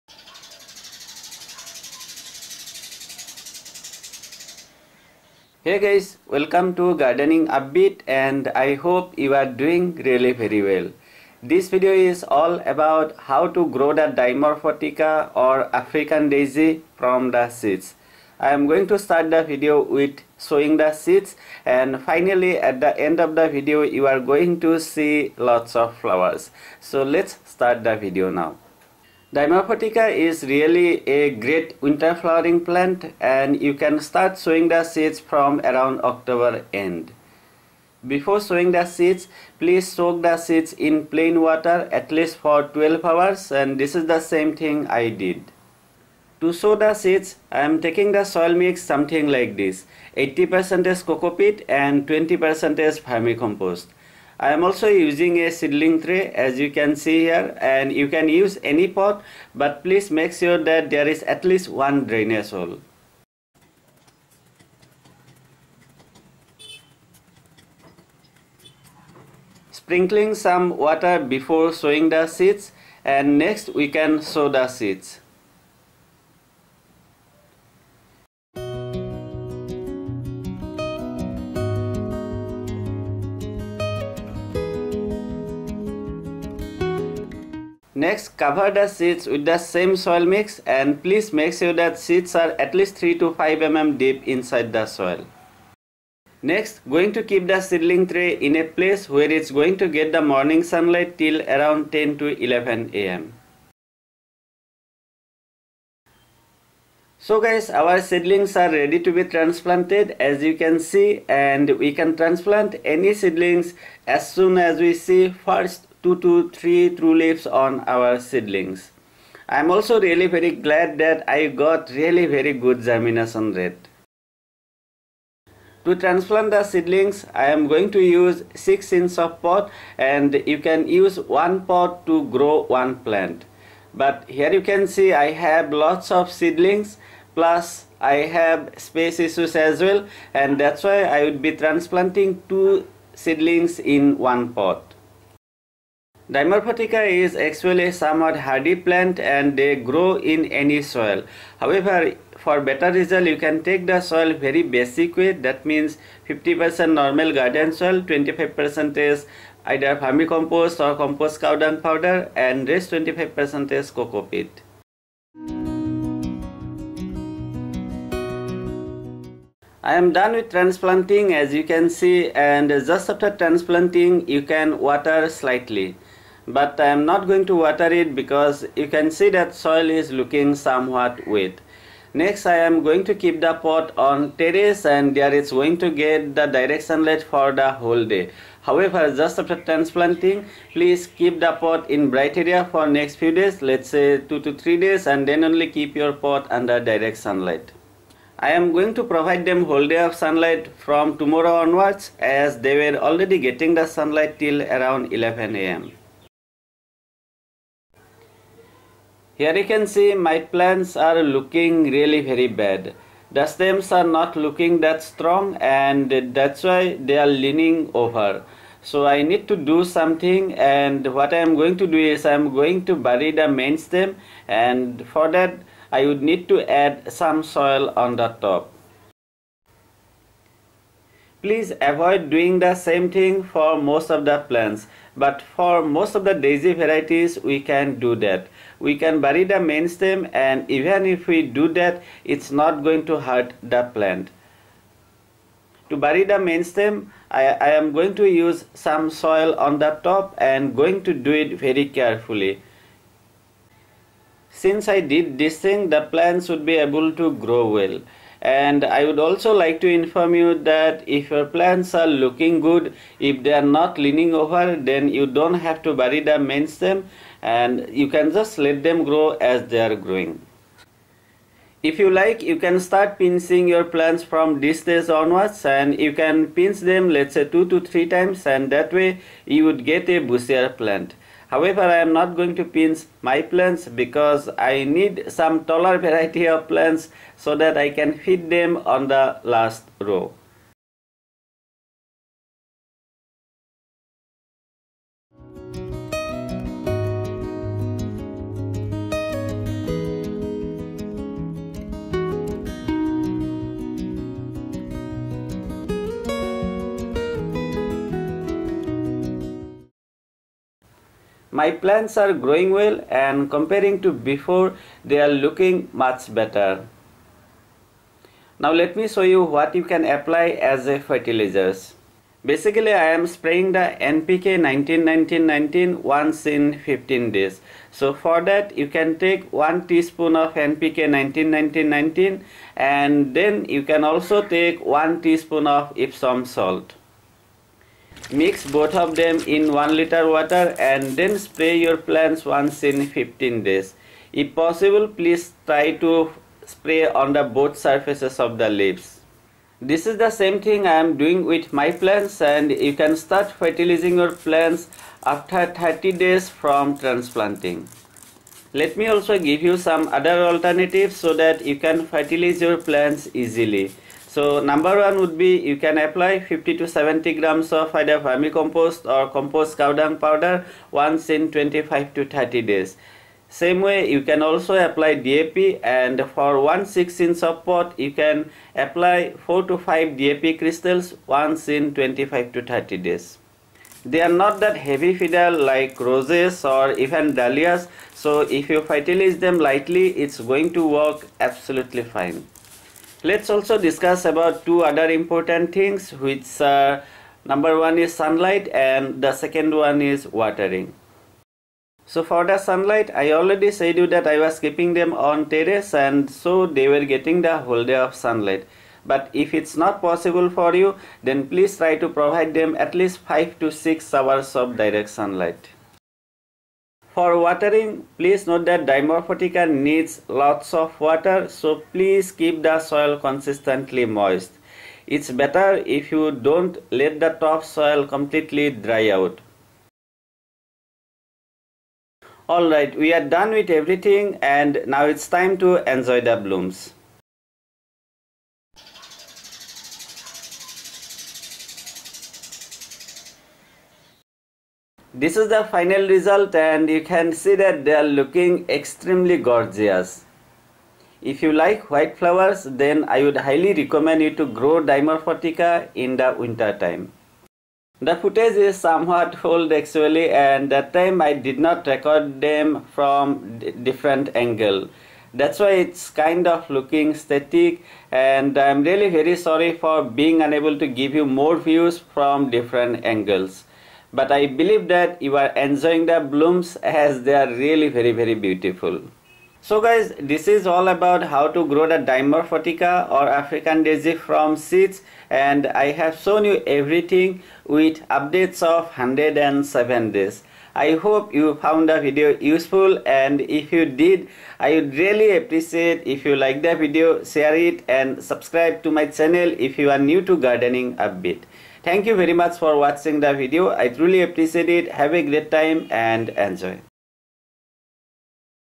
How to GROW Dimorphotheca/African DAISY From Seed
How to Grow Dimorphotheca/African Daisy from Seed - Dimorphotheca is absolutely gorgeous winter flowering plant and if you love White flowers, I would highly recommend you to grow Dimorphotheca at your home. Here in this video I have covered everything from sowing seeds to flowering and I spent 108 days so that I can cover complete life cycle in a single video.

I hope you would enjoy the video and if you have any questions at all, please let me know and I would get back to you ASAP. Thank you very much for watching and Happy Gardening :-)